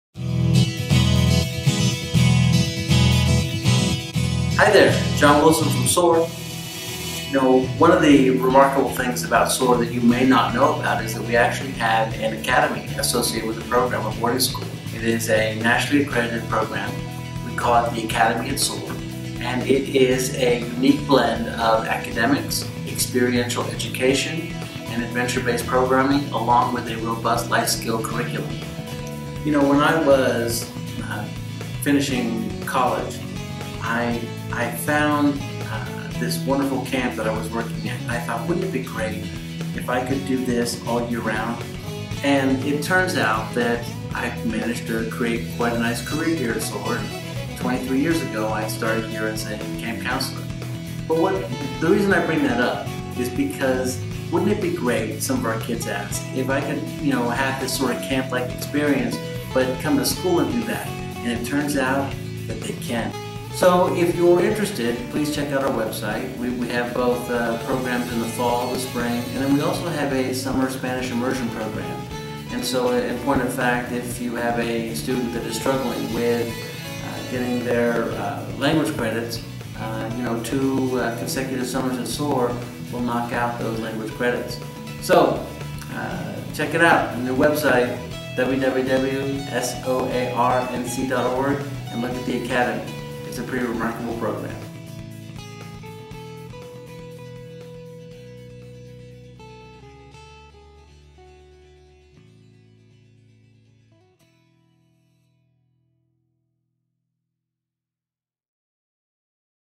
The Academy at SOAR
Today John talks about the nationally accredited Academy at SOAR, and how we provide individualized education with outdoor and life skills components!